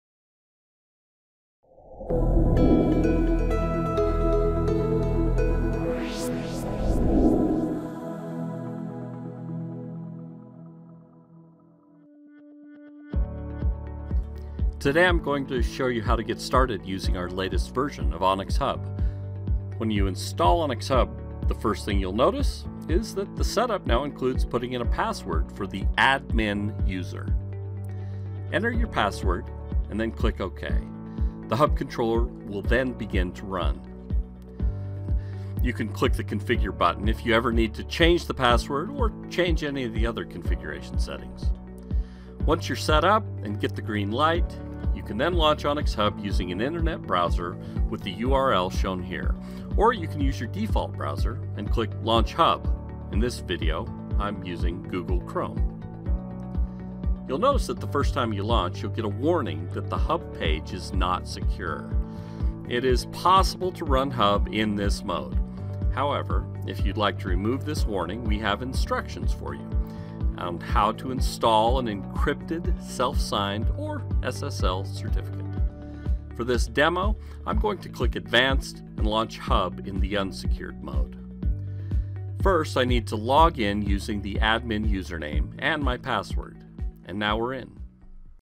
Getting started with ONYX Hub 2.0
The ONYX Hub controller to launch ONYX Hub, the first login screen, SSL certificates and the home page for a site-wide snapshop of the entire print production site data